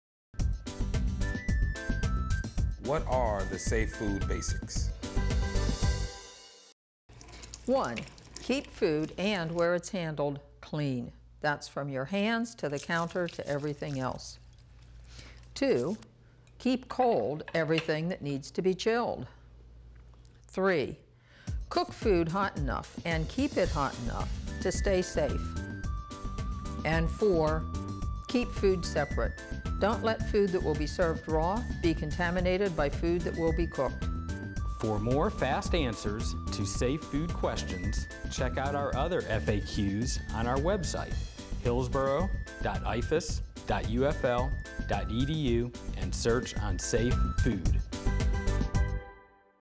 Safe Food FAQs -- What Are The Safe Food Basics?
This introductory video presents the four basic rules of safe food handling - hot, cold, clean and separate.
Producer and Scriptwriter, Mary Keith; Videographer, Graphics and Editing, Billie Lofland; County Extension Director, Stephen Gran.